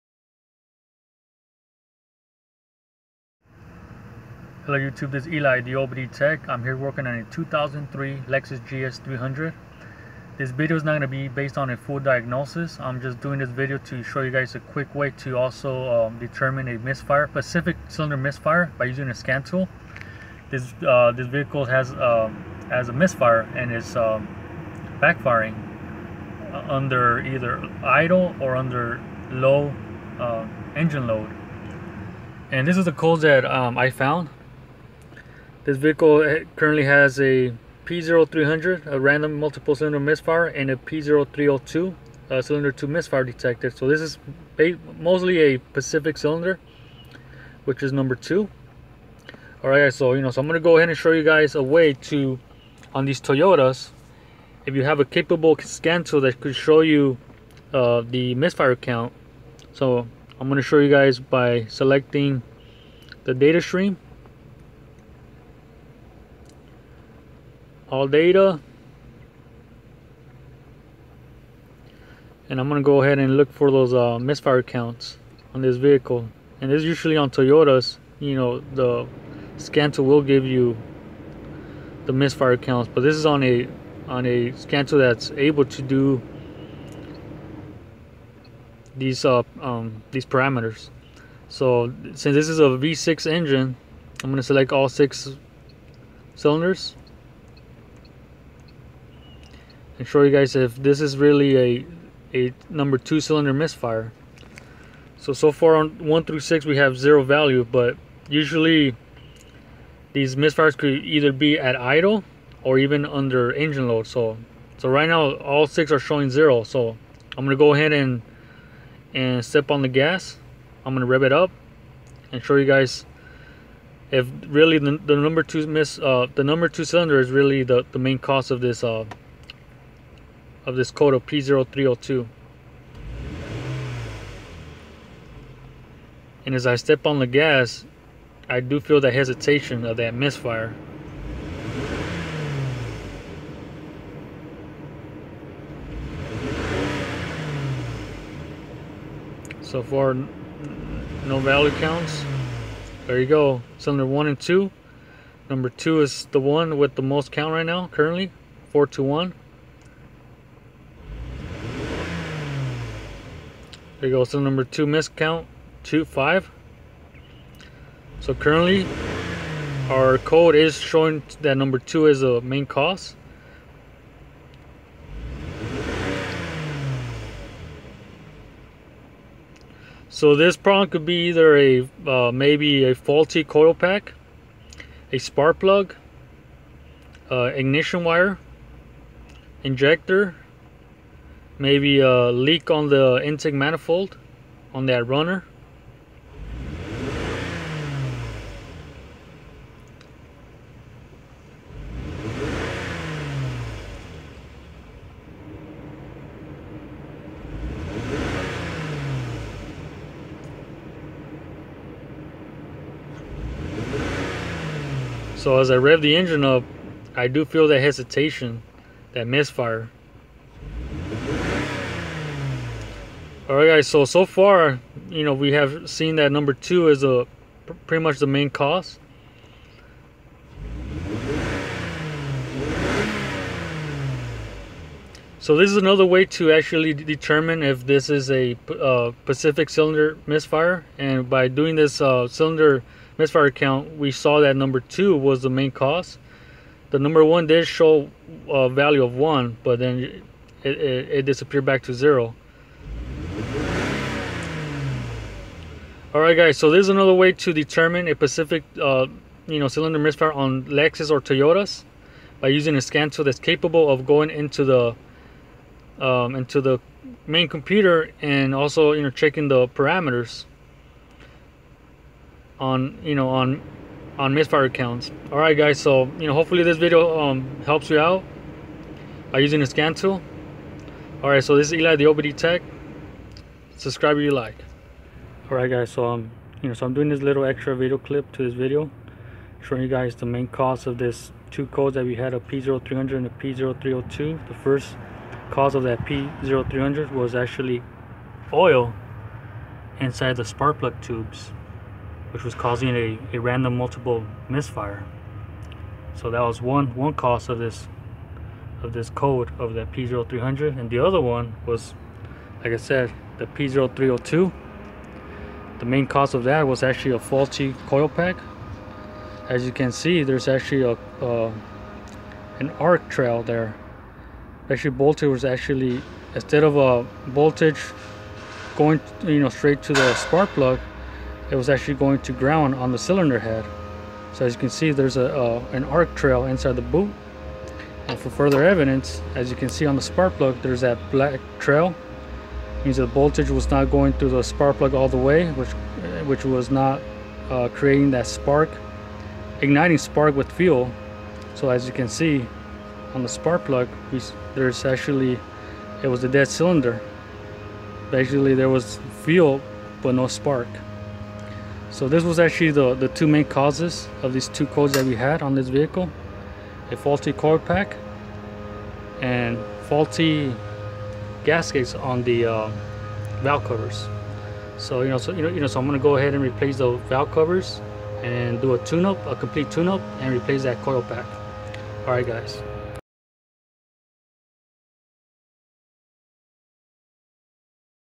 (Toyota/Lexus) Using Scantool Misfire Counts
2003 Lexus GS300 V6

In this video I am demonstrating that you can use a capable scanner to diagnose a misfire condition by using misfire counts on Toyota/Lexus vehicles.